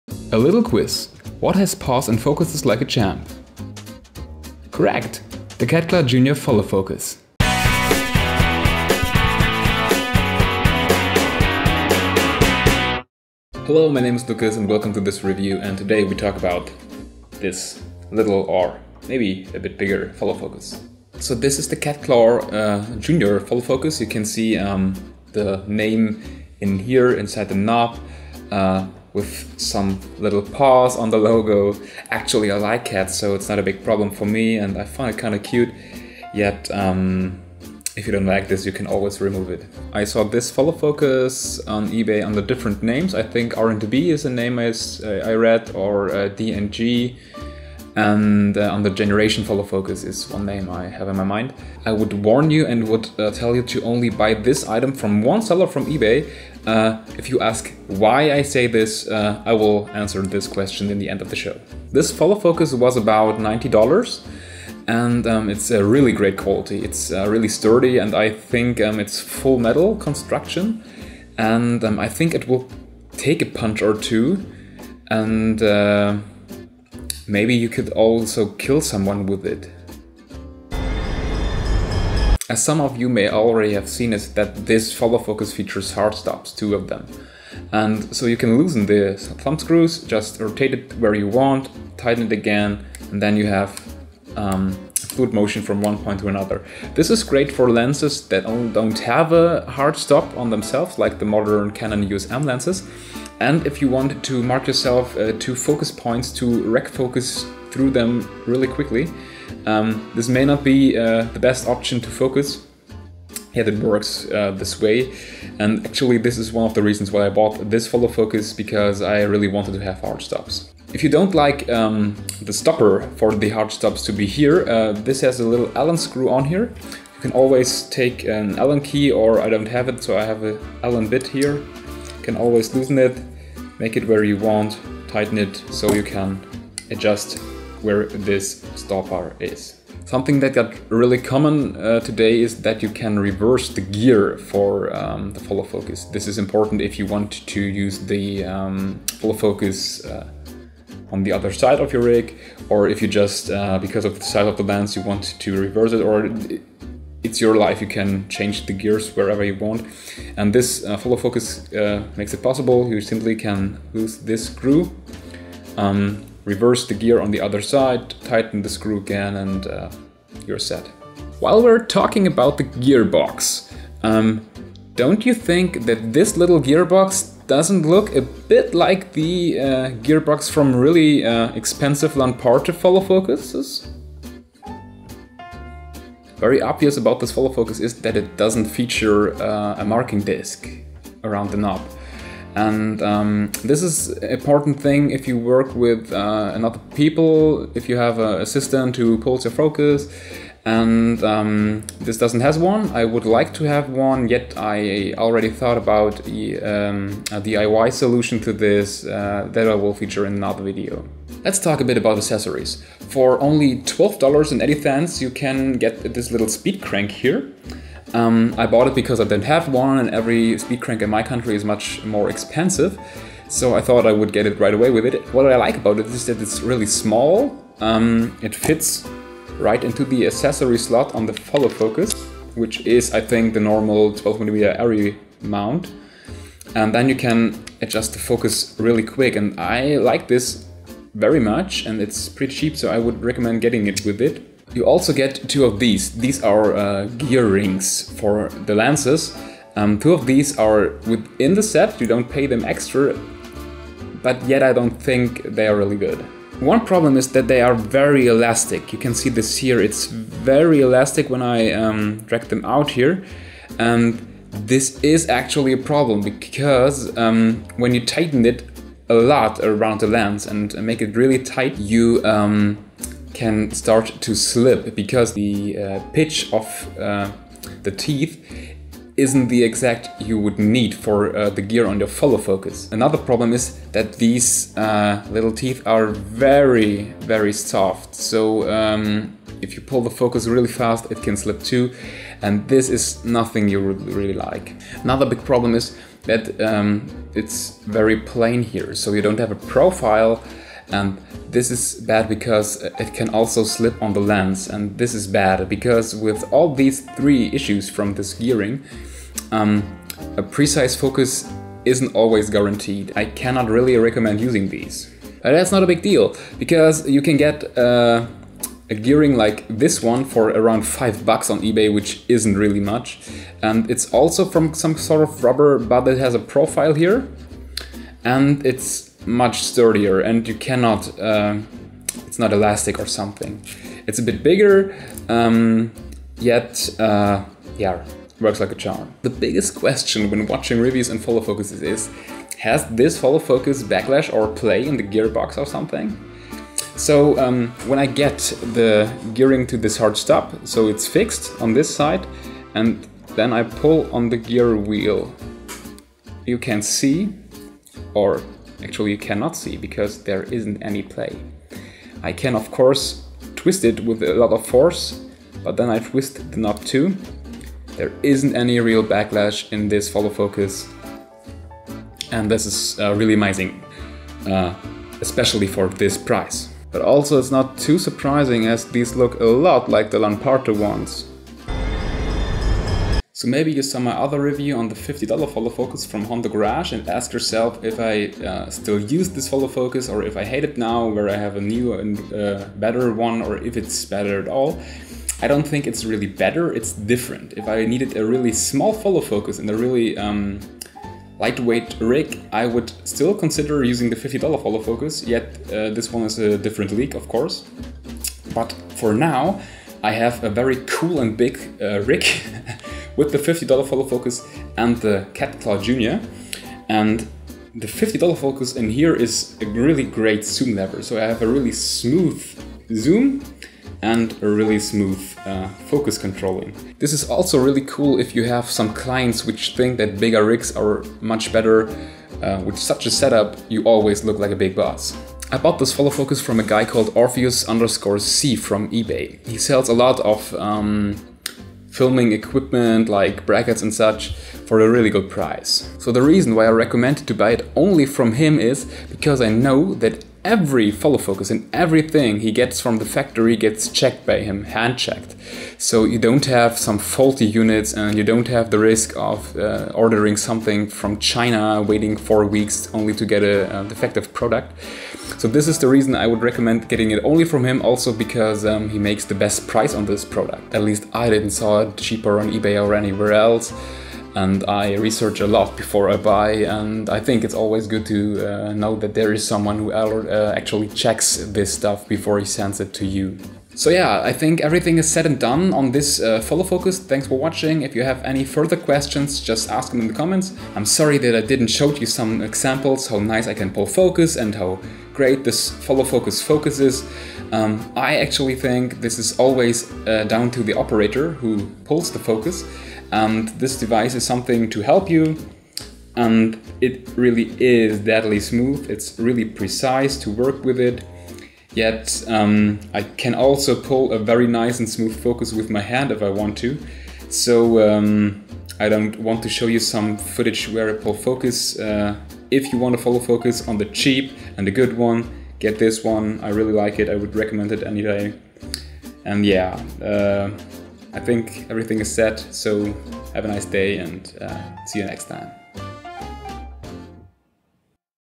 CatClaw Junior Follow Focus (like D&B, Generation) REVIEW
Reviewing the CATCLAW Jr. Follow Focus
Please actiavte Captions (important regarding the Gear-Rings)

When not in store, maybe leave a kind message :)

Any questions, comments or something else? Write it in the comments! :)